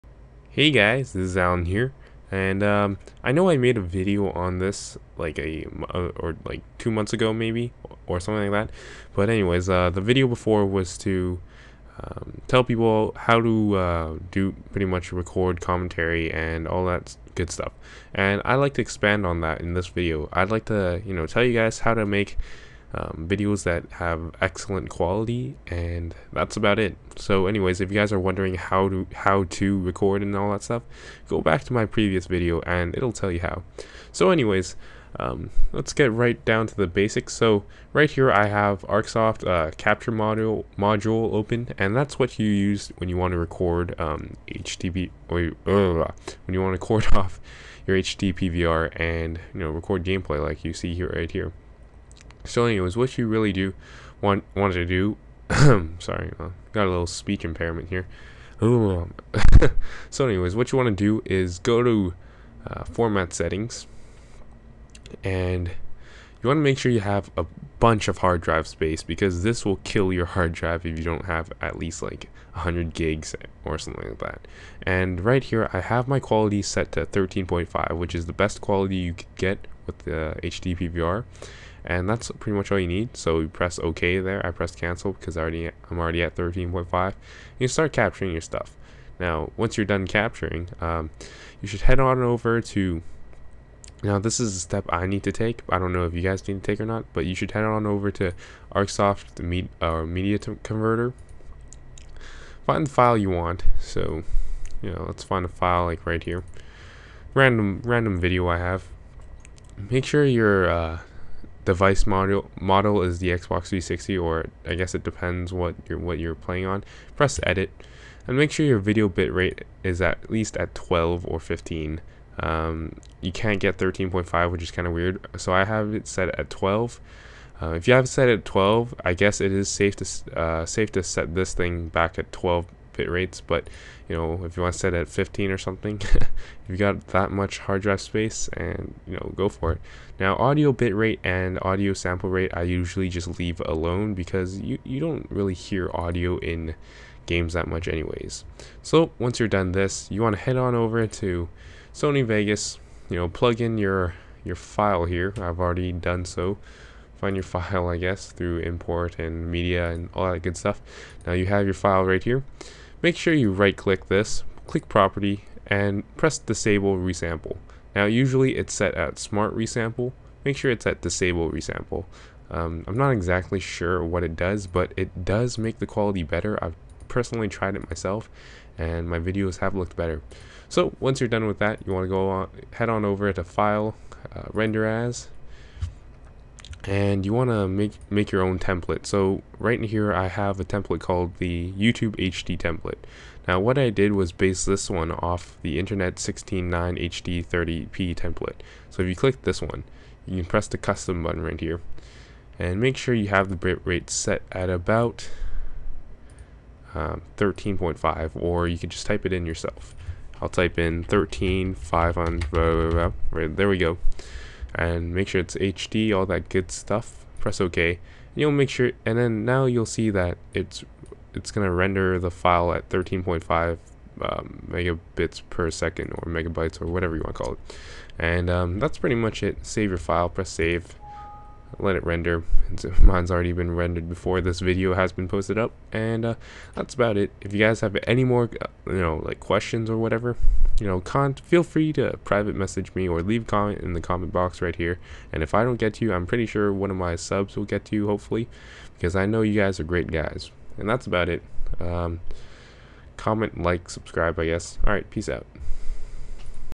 How to: Get Excellent Quality On Your HD-PVR Recordings w/ Sony Vegas (PT.2)
Please "Like" This video as it does help me out quite a bit. Every rating counts!
Wait a few hours for HD to process if it looks crappy.

Just a lil guide on how to get better quality. If you want to watch how to record. Part 1 is here: